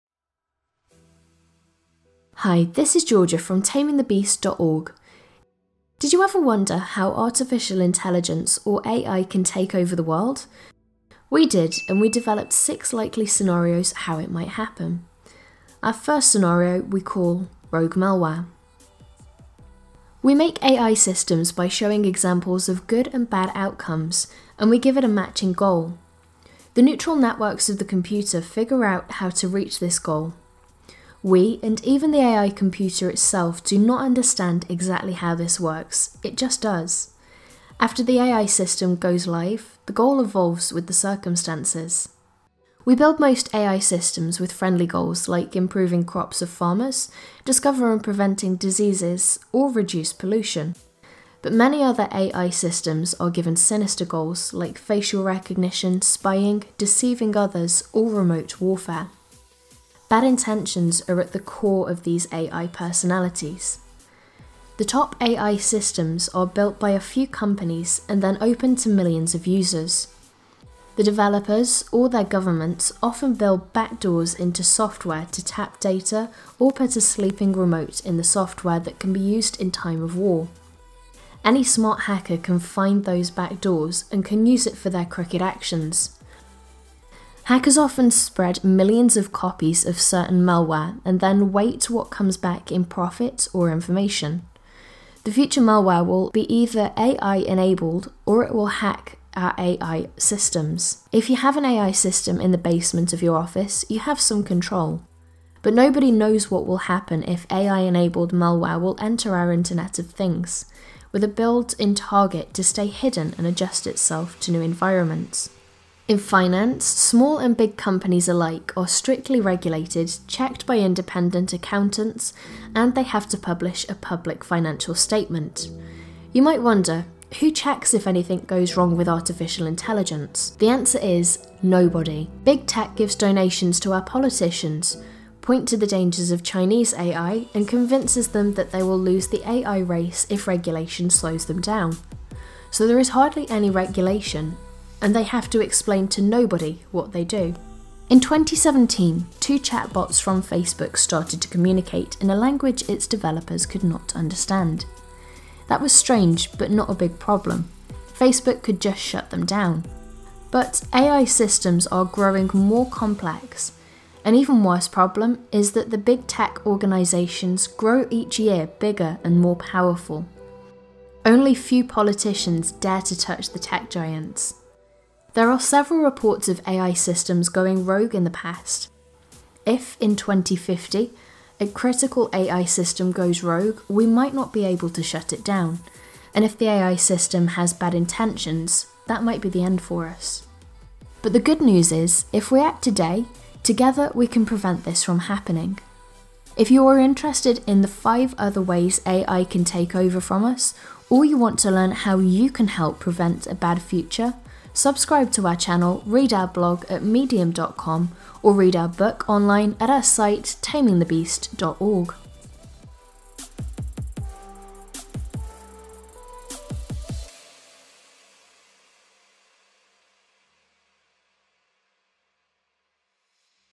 End of World Scenario 1: Rogue AI Malware
Do you think an AI takeover of humanity is on the horizon?

We researched the 6 most likely scenarios how Artificial Intelligence (AI) can take over from humanity.

AI is still a small part of our lives, and nobody expects any serious problems in the coming years. But AI develops further. Now most AI applications can do only one thing, like curing cancer or translating. The coming generation of AI can de multiple tasks exponentially better than us.

An AI-enabled future can bring us enormous prosperity. But there is also the risk that just a few companies or governments control the AI systems to their advantage. Or even worse: A powerful and independent thinking AI machine decides not to listen any more to us.

We explored the six most likely scenarios, how this exactly might happen.

If we know how we can lose control, we can take measurements to prevent this from happening.

The goal of is to create awareness of the existential risks of AI and to developed a set of solutions that can keep the future safe for our children.

Artist - Dávid Kakucs
Editing - Ervin Szabó
Voice - Georgia Turner
Idea and script - Remy van Nieuwenhoven